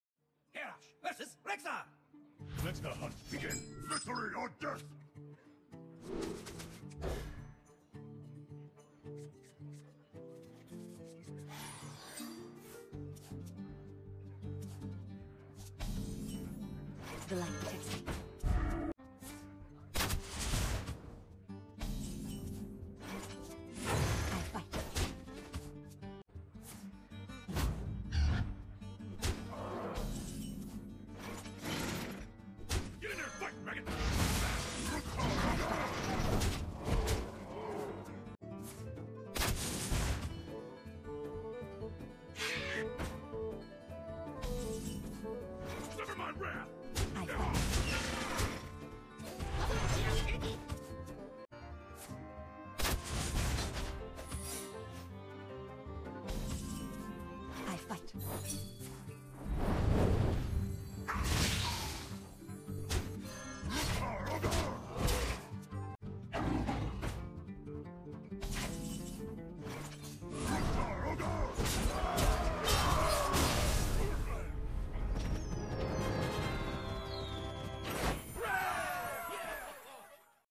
Legend Deck: Avalanche Warrior E05 (vs Hunter)
We are just getting into the habit of showcasing gameplay videos along with the decklist. Please provide any suggestions/feedback that you may have!

The videos have been sped up and dead air edited out for your viewing pleasure. Please don't forget to leave any feedback/suggestions you may have for future videos!